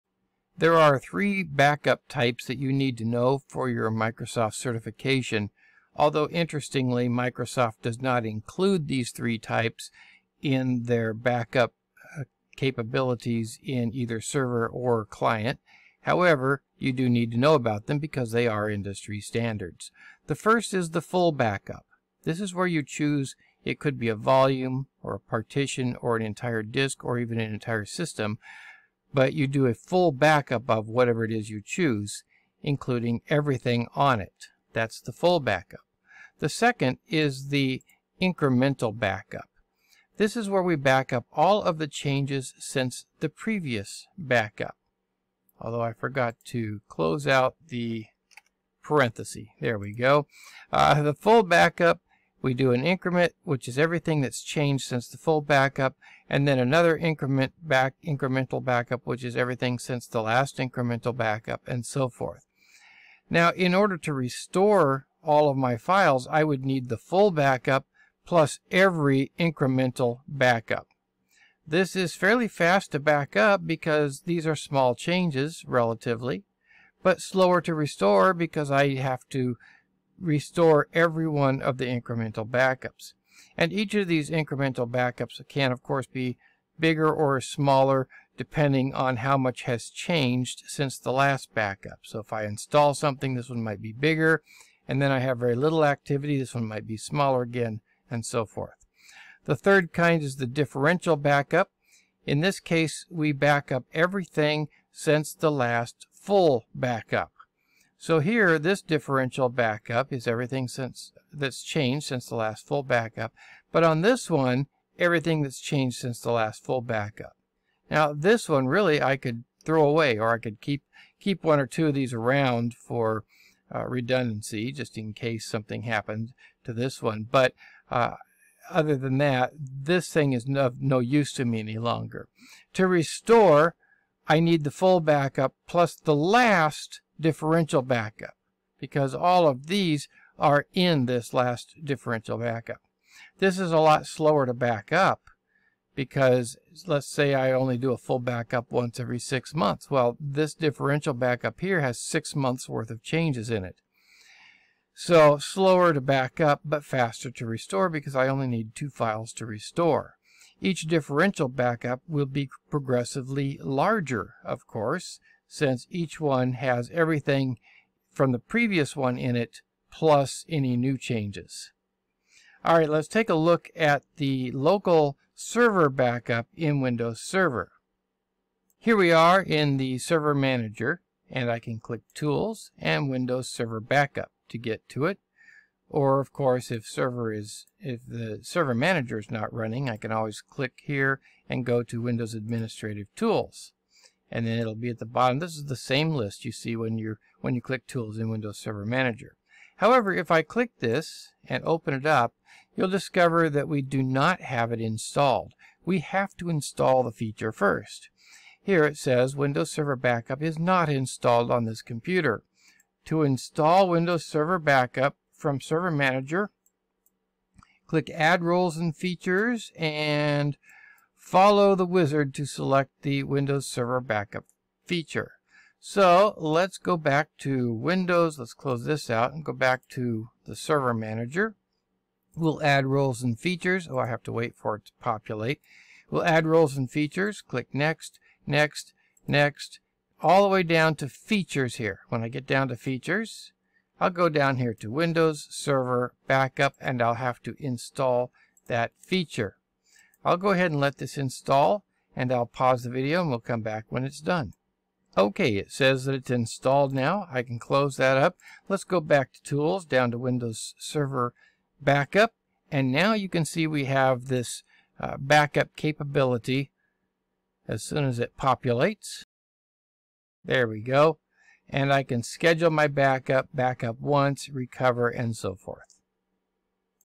Three types of backup
This video is part of a course designed for the MTA certification for Windows Server Fundamentals. As such it s not an in-depth treatment of the topic, but represents the level of understanding needed for certification.